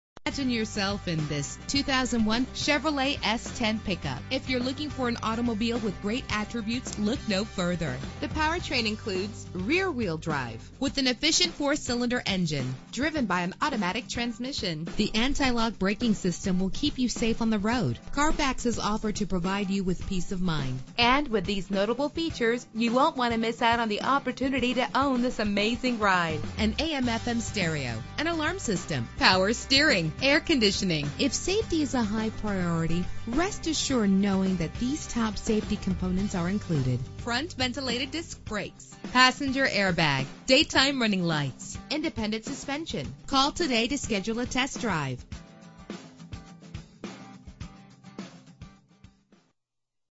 2001 Chevrolet S-10 Pickup - Pensacola FL
Year: 2001
Make: Chevrolet
Model: S-10 Pickup
Trim: 123 WB
Engine: 2.2 liter inline 4 cylinder 8 valve
Transmission: Automatic
Color: Light Pewter Metallic
Mileage: 83191
Address:
230 Beverly Parkway